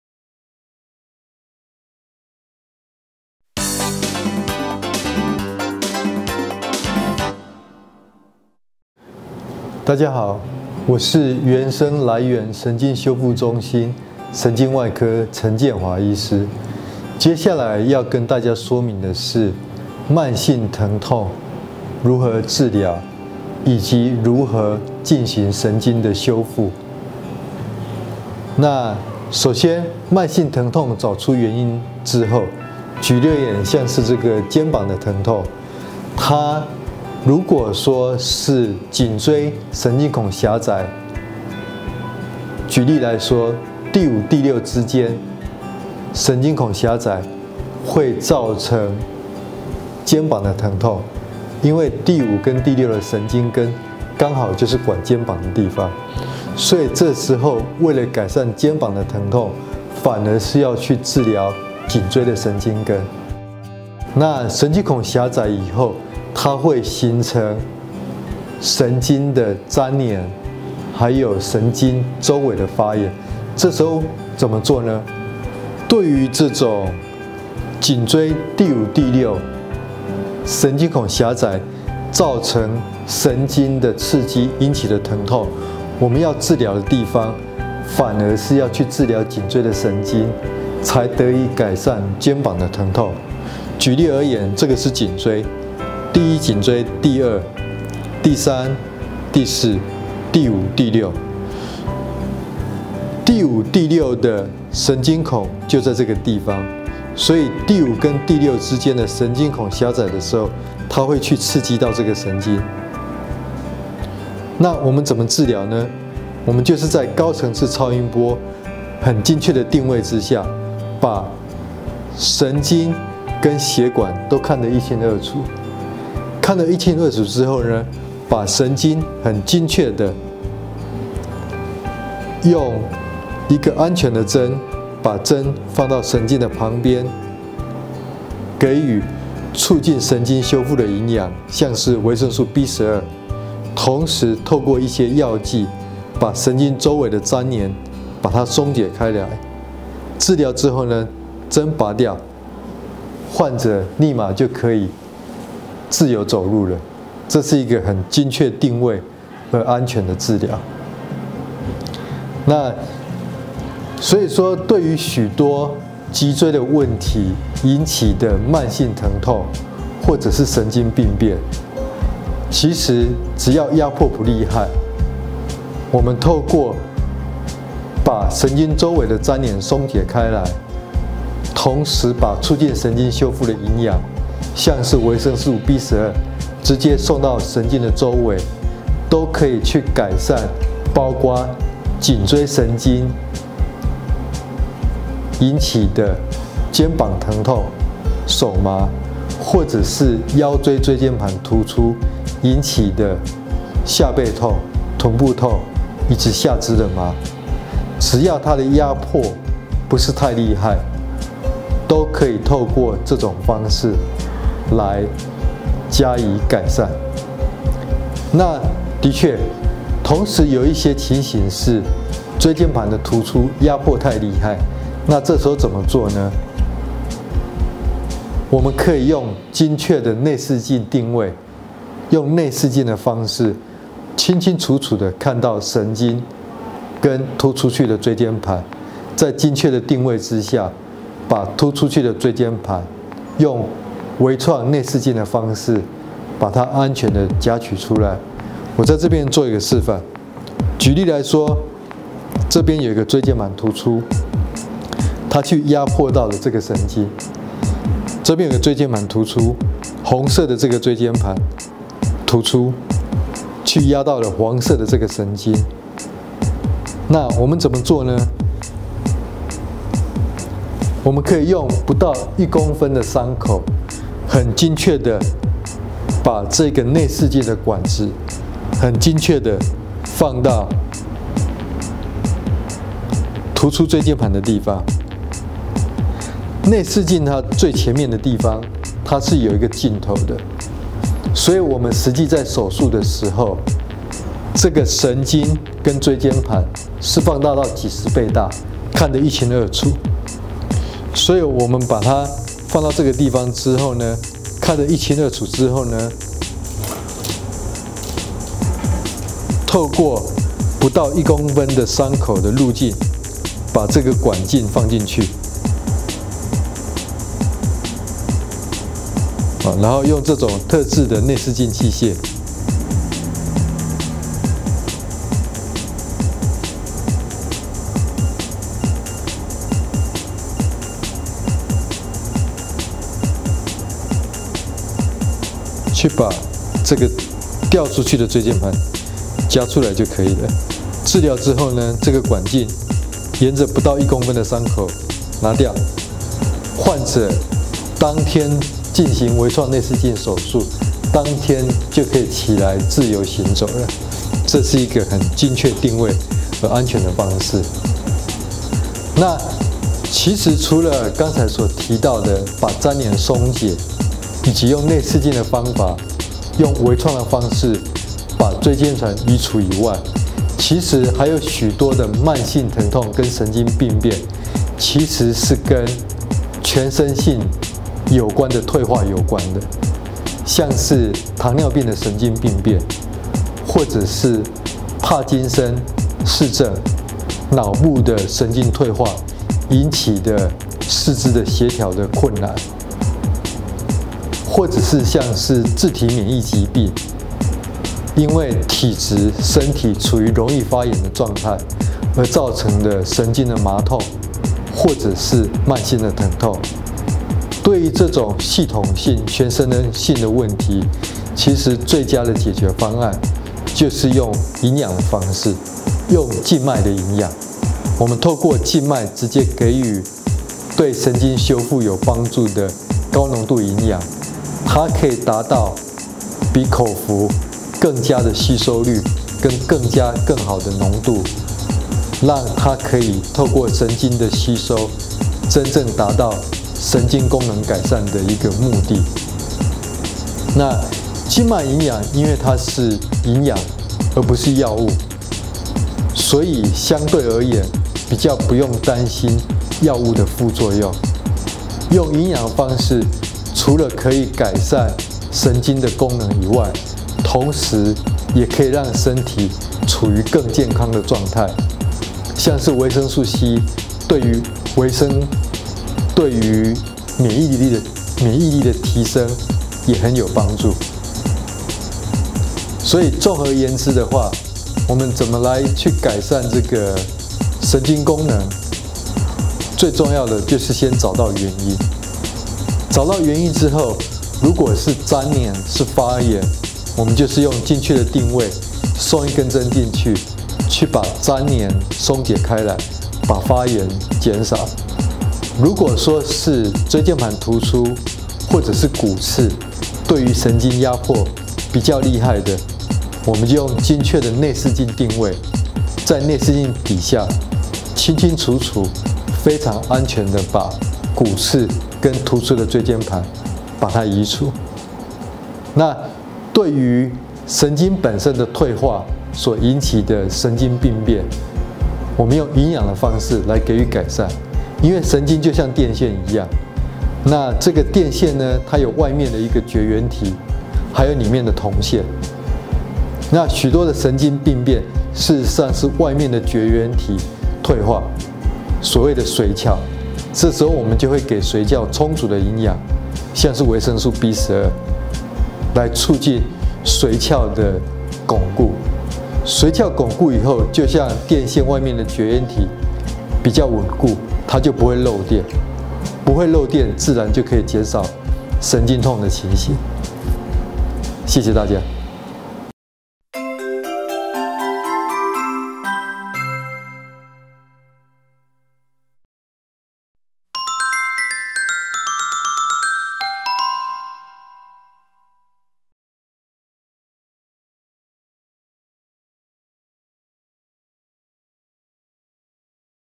慢性疼痛如何治療? 如何進行神經修復? 陳建華醫師講呼您知
20210408
認識厝邊好醫師~神經外科 陳建華醫師

現任職務:
員榮醫療體系員生醫院萊源神經修復中心 醫師

學經歷:
台灣大學醫學系
台灣大學醫學工程研究所博士班
美國加州大學洛杉磯分校神經調控及疼痛治療臨床進修
台灣大學醫學系
台中榮總神經外科主治醫師
美國加州大學落杉磯分校神經調控及疼痛治療臨床進修
台中榮總神經外科總醫師
神經外科專科醫師
台灣疼痛醫師專科醫師

主治項目:
萊源神經修復術、高層次超音波導引疼痛治療、腰椎微創內視鏡手術